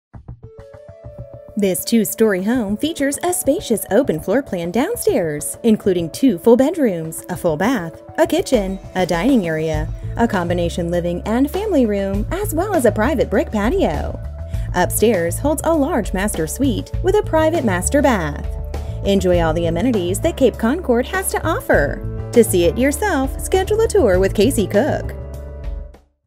481 Nantucket Glen Escondido, CA
481 Nantucket Glen, Escondido, CA

Dont miss this move-in ready, condominium-style home in Cape Concord. The location of this home is wonderful and is close to everything including schools, grocery shopping, entertainment, freeways and public transportation. This two-story home features a spacious open floor plan downstairs includin